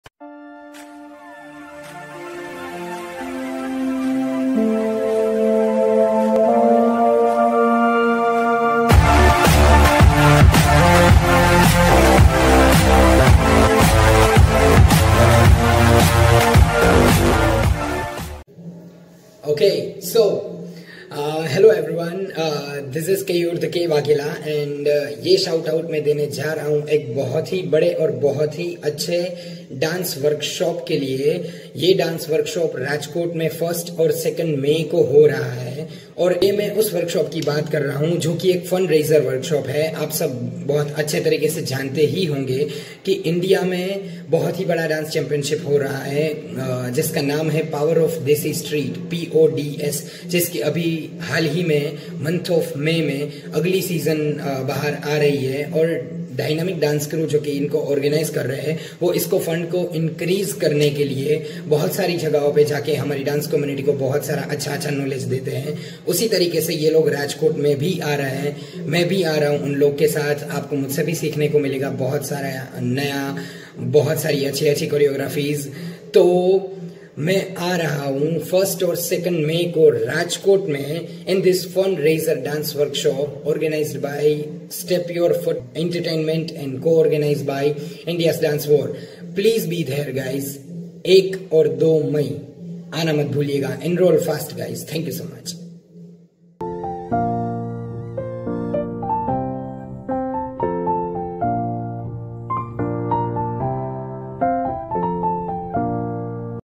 Keyur { THE K VAGHELA } | @P.O.D.S. RAJKOT DANCE WORKSHOP | @S.U.F. ENT.
Here Are the video bites of " Keyur The K Vaghela ".

Share the link guys for promorting the event name.

       P.O.D.S. RAJKOT DANCE WORKSHOP.
     THE FUND RAISER DANCE WORKSHOP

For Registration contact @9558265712